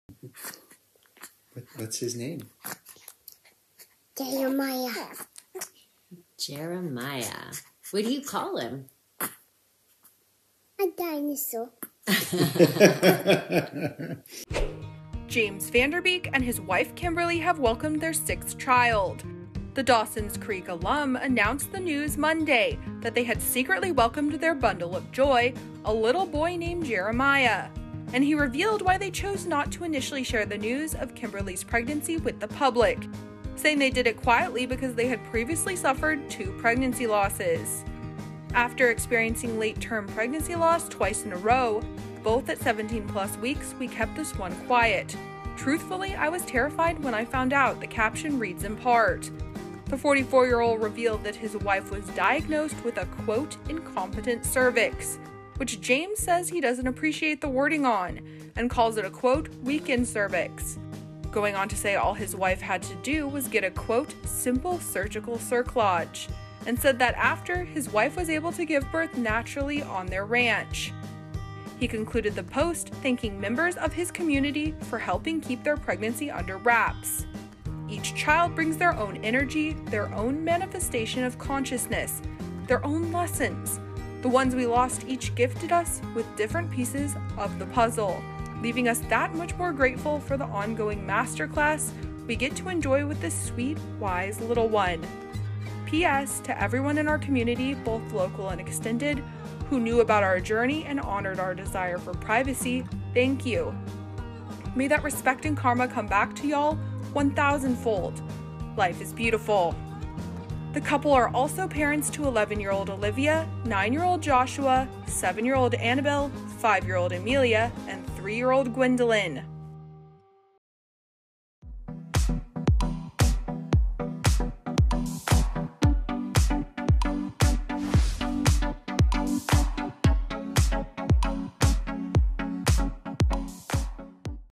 James Van Der Beek & Kimberly Secretly Welcome Baby No. 6
JameJames Van Der Beek and his wife Kimberly have welcomed their sixth child! The "Dawson's Creek" alum announced the news on Monday that they had secretly welcomed their bundle of joy, a little boy named Jeremiah, and he revealed why they chose to not share the news of Kimberly's pregnancy with the public. "After experiencing late-term pregnancyloss twice in a row (both at 17+ weeks), we kept this one quiet. Truthfully, I was terrified when I found out," the caption reads in part.